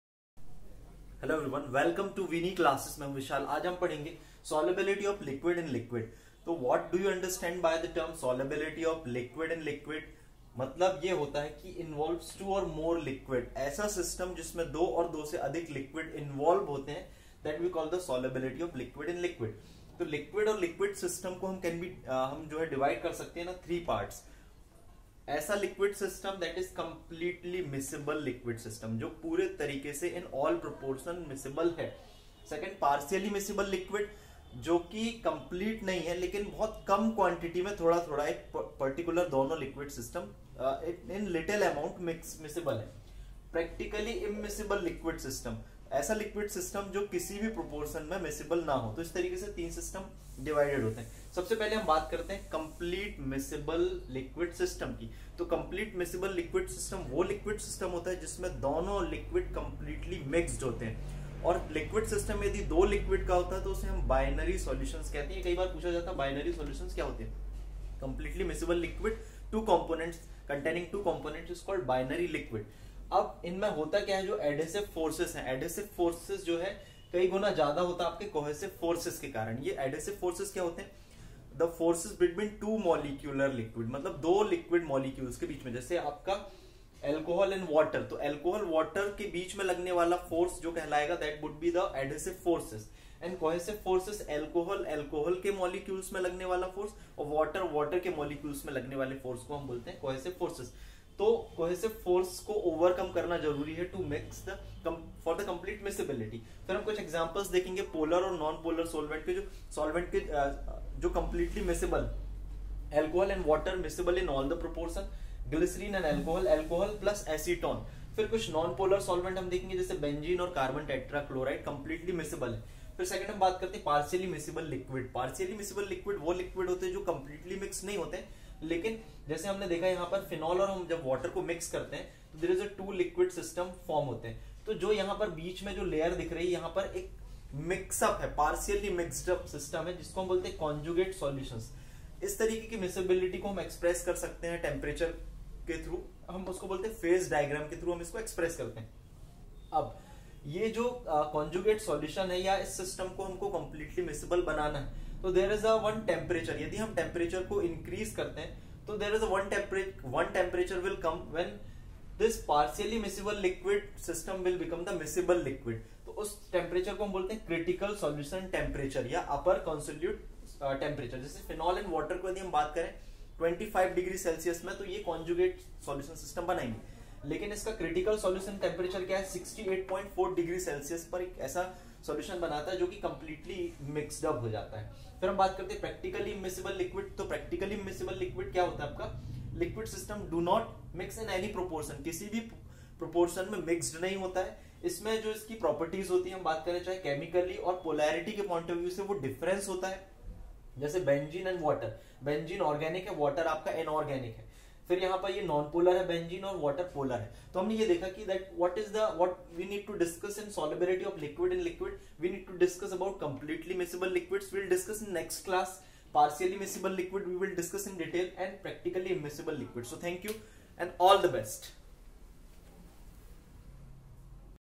Solubility of Liquid in Liquid, Part 16, Physical Pharmaceutics III SEM
Solubility of Liquid in Liquid, Part 16, Physical Pharmaceutics III SEM Binary Liquid and Critical solution Temperature